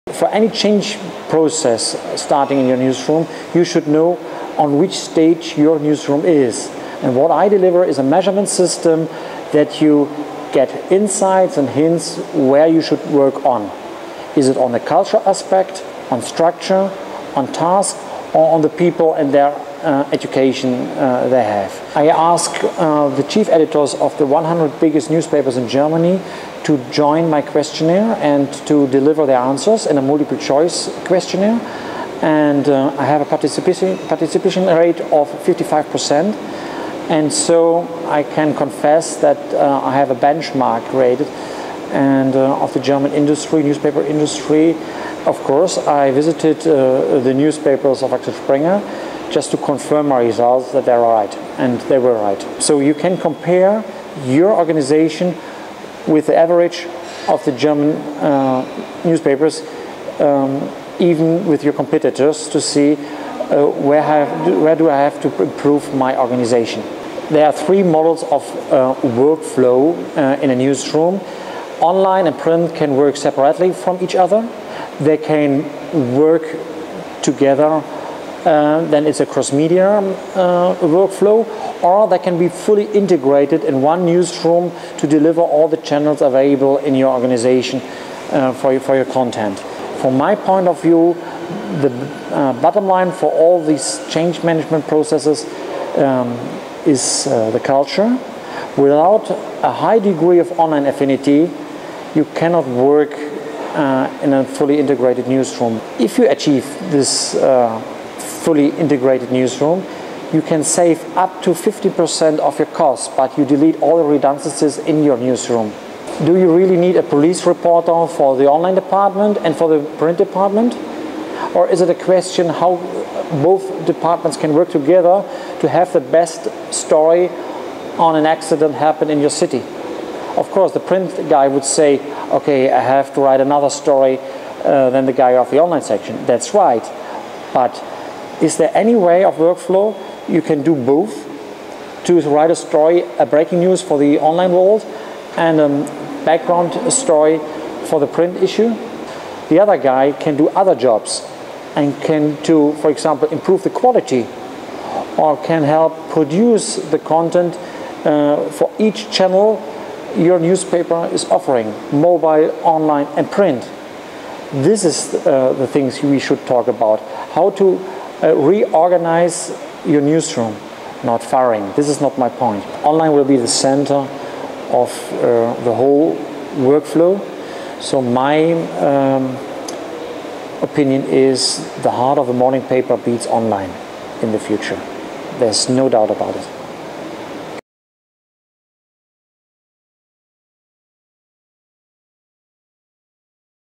INMA Europe 2011: Pit Gottschalk - how much transformed is your newsroom?
Pit Gottschalk, Head of CEO Office in Axel Springer (Germany), explains his measurement system of the newsroom transformation process. He studied over 50 newspaper newsrooms in Germany and prepared report helping media managers to compare their improvements with other players on the newspaper market. Now Pit Gottschalk is rolling out the analysis to other countries. If you would like to be involved in the survey, you can contact CEO Ofiice of Axel Springer in Germany.

Interview recorded on INMA European Conference in Cascais, Portugal, 19-21 October 2011.

Pit Gottschalk was interviewed by Artur Karda from 112MEDIA -- consulting, training and outsourcing for media.